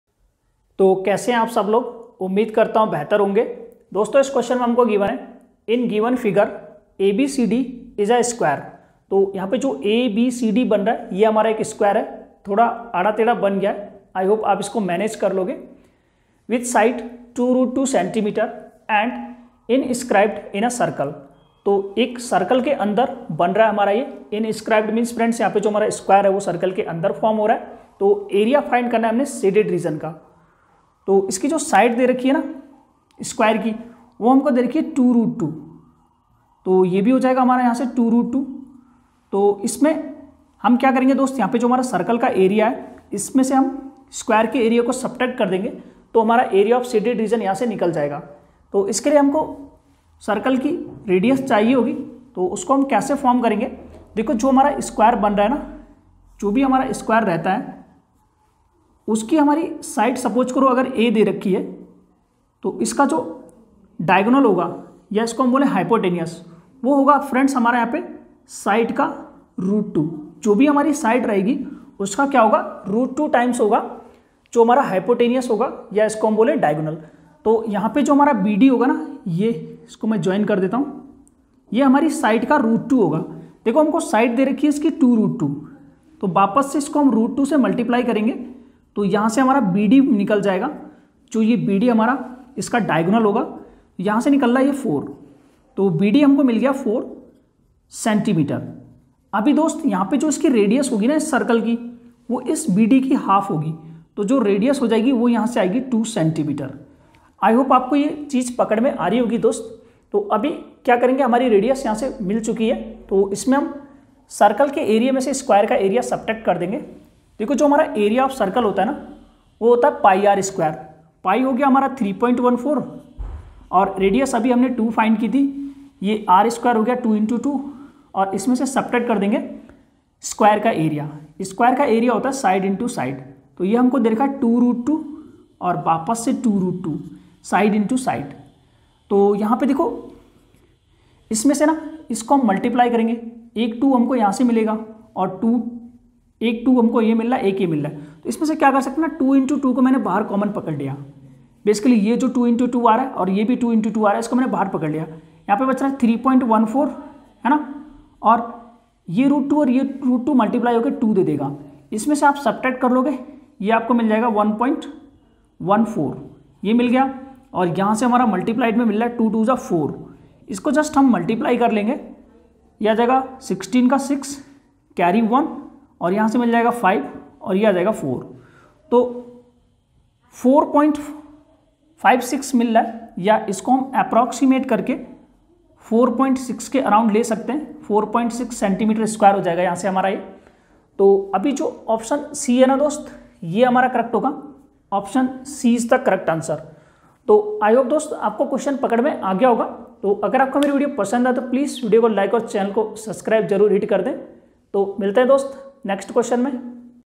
in given figure ABCD is a square with side 2√2 cm and inscribed in a circle the area of the shaded
HOTS123in given figure ABCD is a square with side 2√2 cm and
inscribed in a circle. The area of the shaded region is:-
This video is based on the new syllabus  and all the questions are very important for the exam point of view.